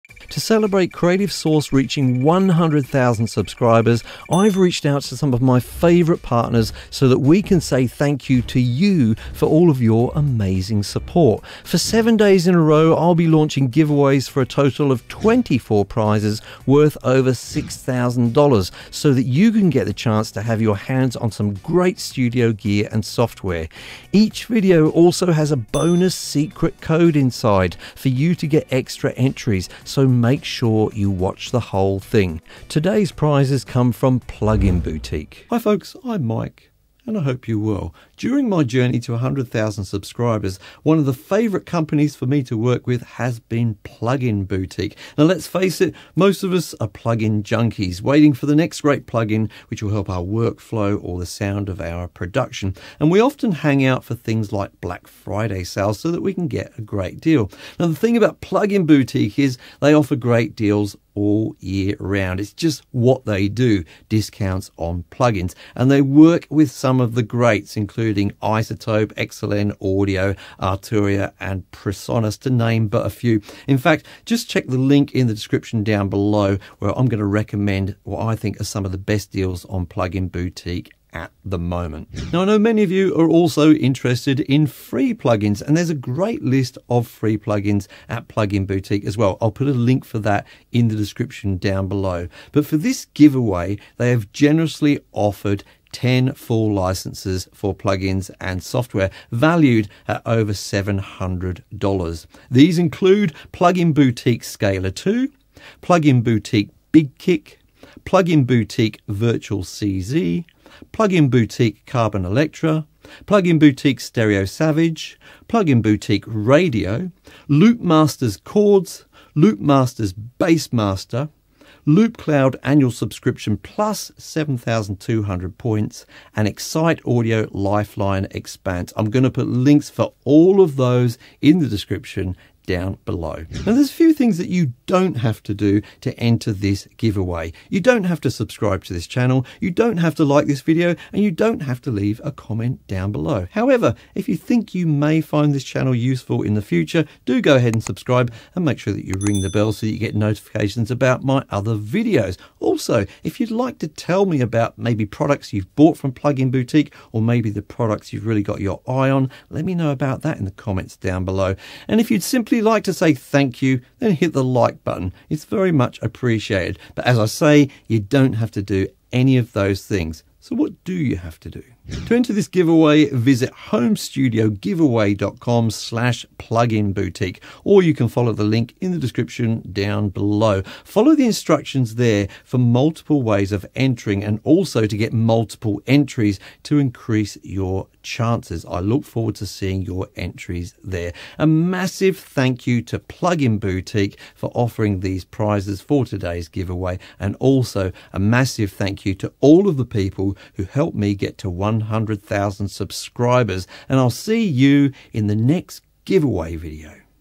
7 Days of Giveaways - DAY 3 - Plugin Boutique
In day 3 of "7 Days of Giveaways", we have awesome prizes from Plugin Boutique!